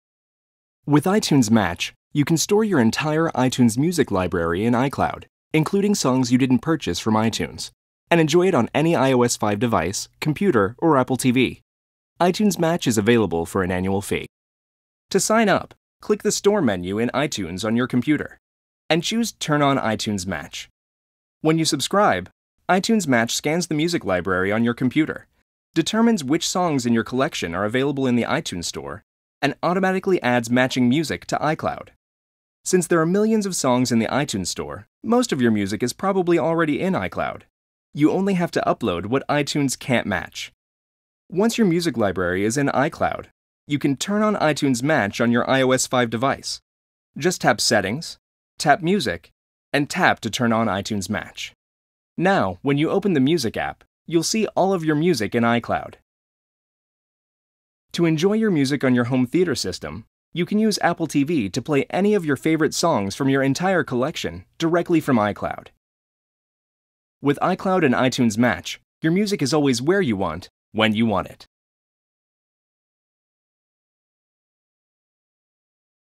Apple — How to setup iTunes Match?
Apple has posted their own walkthrough video that shows how to setup iTunes Match on iTunes on the Mac and then how to access your music on an iOS device like an iPad or Apple TV.